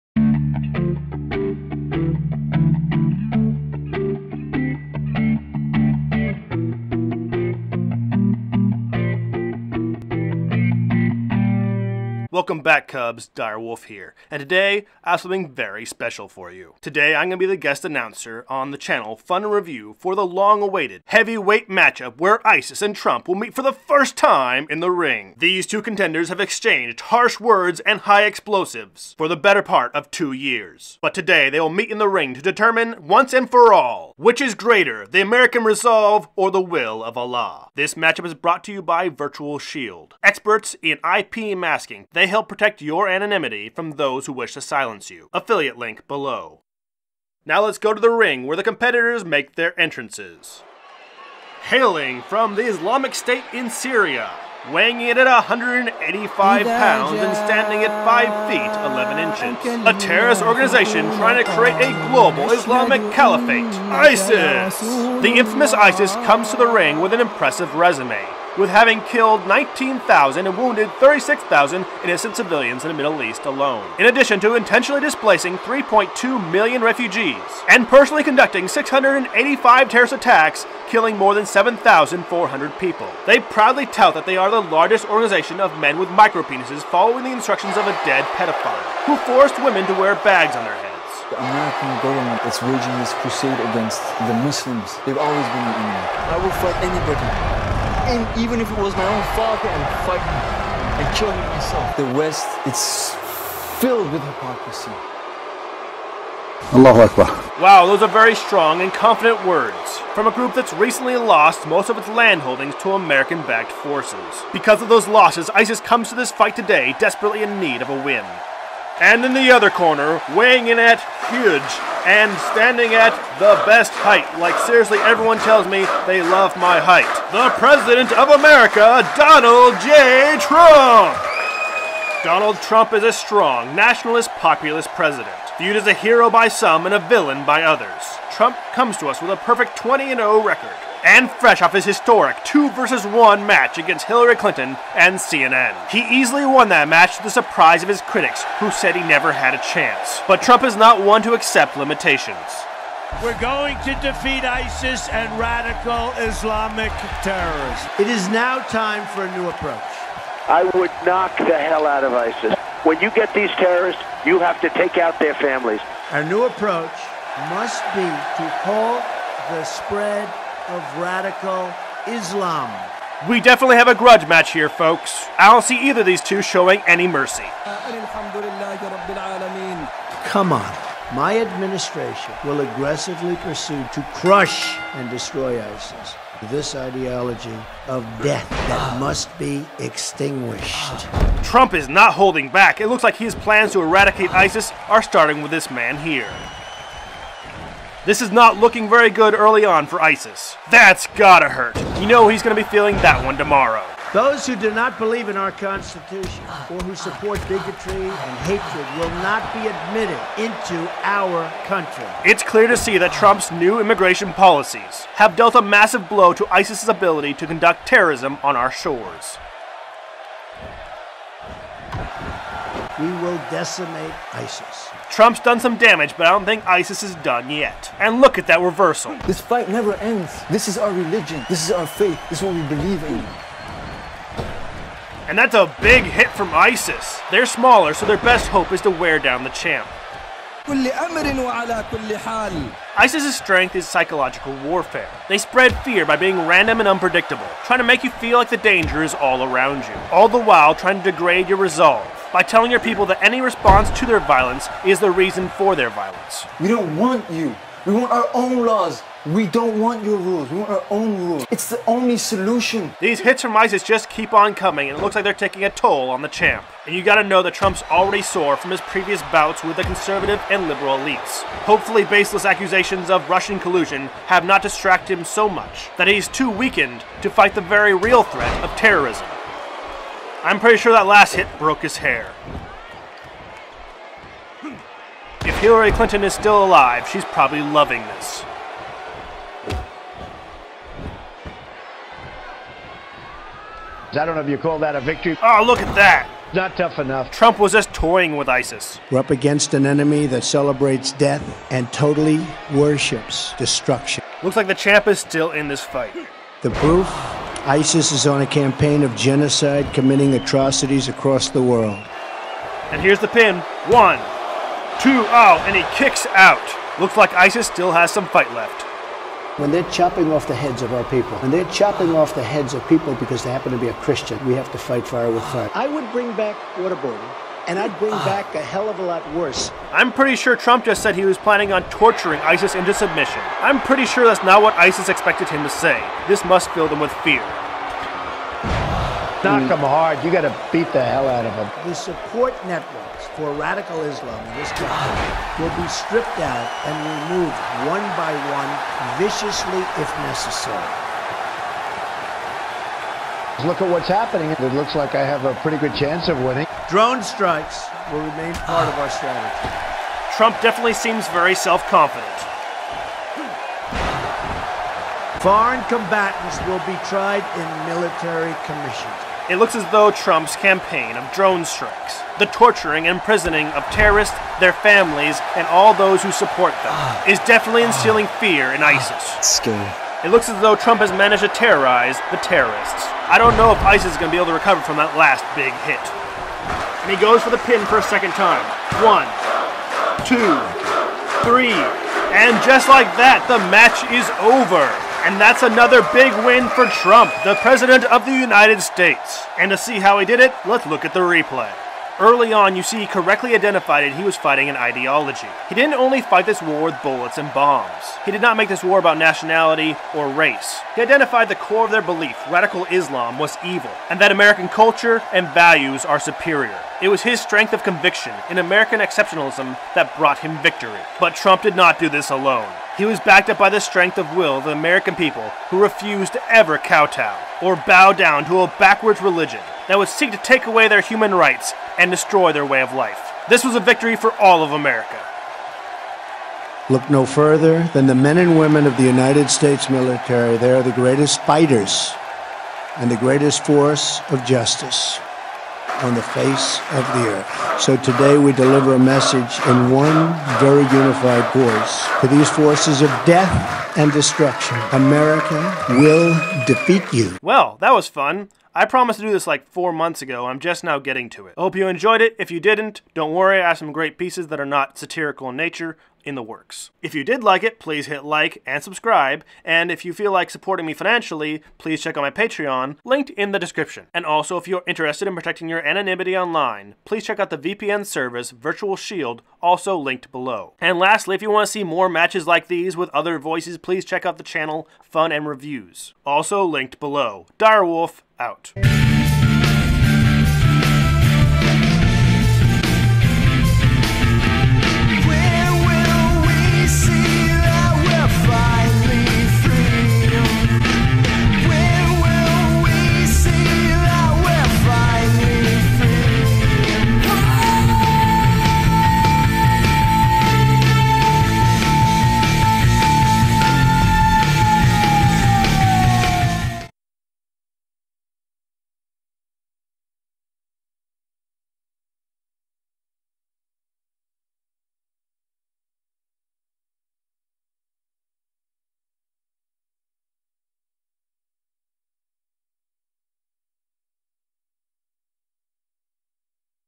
TRUMP KILLED A MAN featuring Dire Wolf
Dire Wolf guest Commentates as President Donald Trump takes on Isis one on one.

special thank you to Dire Wolf.

info for Dire Wolf.
Mic: Blue SnowBall
Comp: IMAC
Software: IMOVIE